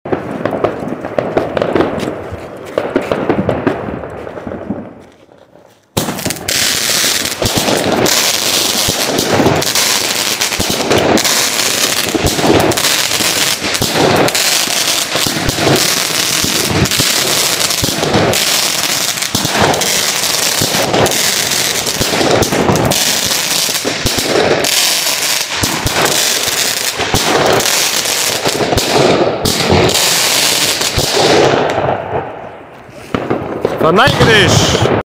WECO PYROCK...2 Stufen Crackling Batterie... SILVESTER 2021/22
Ein kleines Shortvideo, da es leider versehentlich im Hochformat aufgenommen wurde. Dieser Artikel von 2020 sollte  im Lidl  unter der Labelung The New Beastmaster erhältlich sein, was nun wegen des Abgabeverbotes nicht der Fall war. Von meiner Seite her eine Top Batterie.